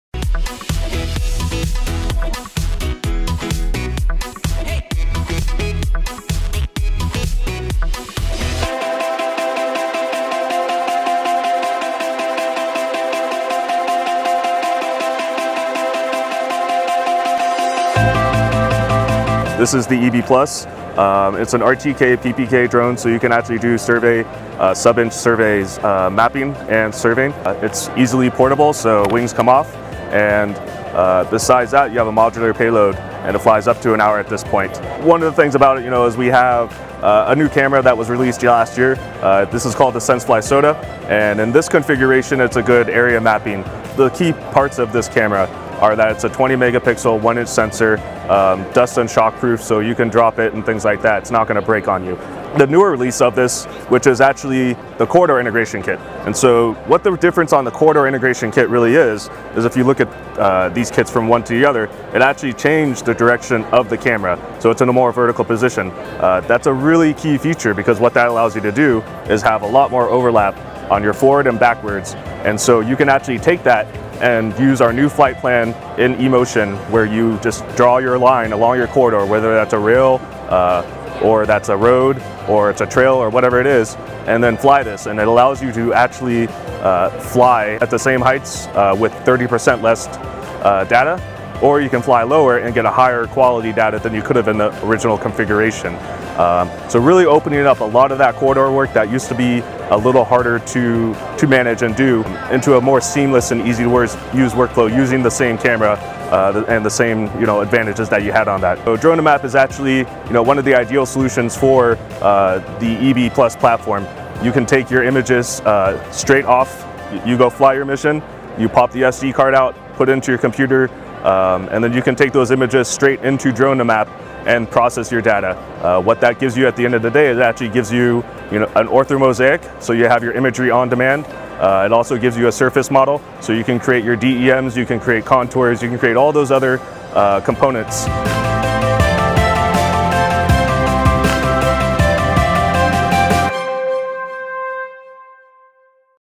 eBee Plus takes survey grade mapping to new a level
SenseFly Briton Voorhees Sales Engineer of senseFly explains eBee Plus a drone that takes survey grade mapping to new a level. eBee Plus provides large coverage for optimal efficiency. The Drone can map more square kilometers per flight. It provides high Precision on Demand. eBee Plus includes built-in RTK/PPK functionality.